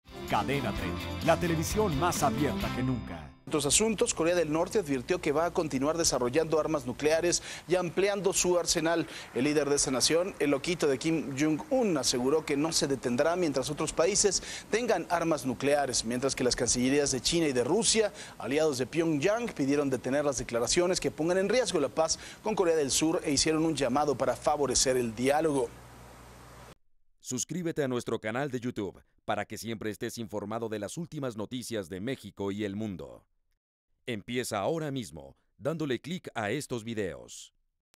Corea del Norte continúa sus amenazas de Guerra
Corea del Norte continúa sus amenazas de Guerra. Corea del Norte continuó hoy con las amenazas sucesivas de esta semana y comunicó que se encontraba en estado de guerra con Corea del Sur.